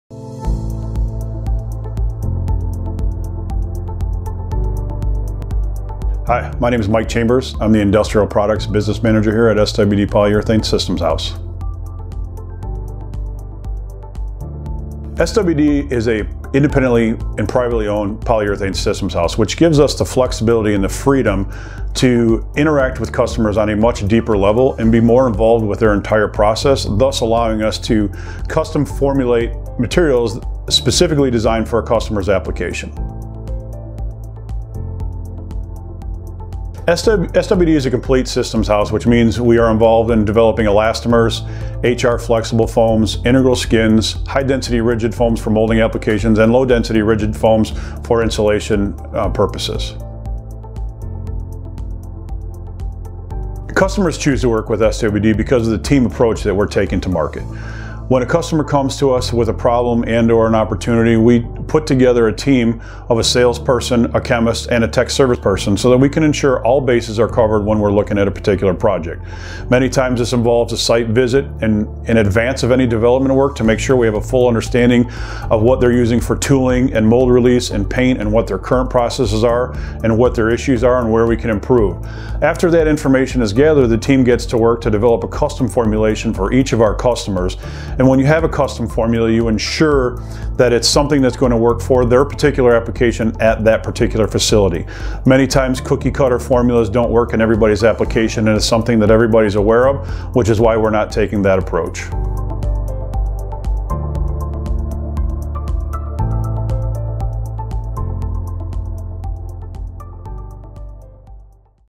SWD's Original Equipment Manufacturing (OEM)
Mike Chambers, Industrial Products Business Manager at SWD Urethane, discusses what makes SWD Urethane different, the types of products offered by SWD, and why customers choose SWD.